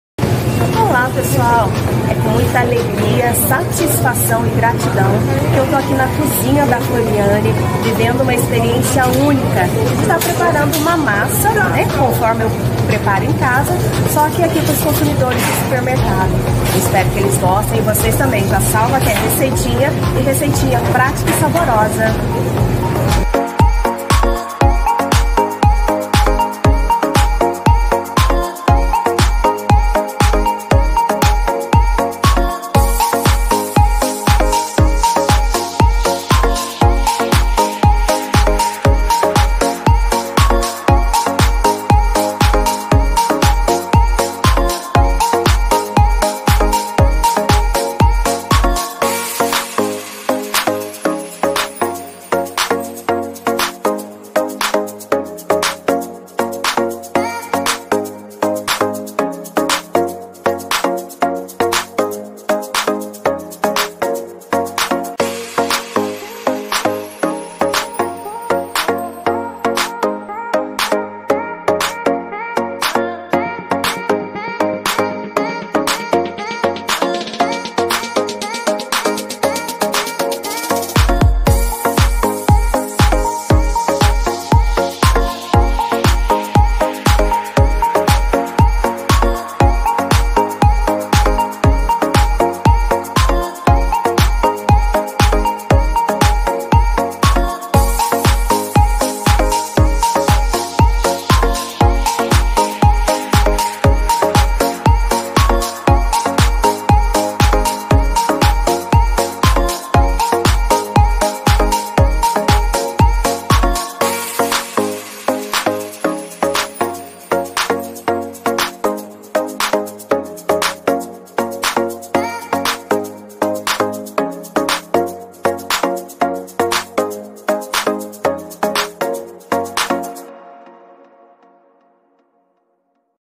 Macarrão Penne ao molho de creme de leite
A @monikkatiane esteve presente em nossa cozinha Floriani Experience e preparou uma receita deliciosa com nossa massa Penne Grano Duro.

Passo a passo e ingredientes no vídeo.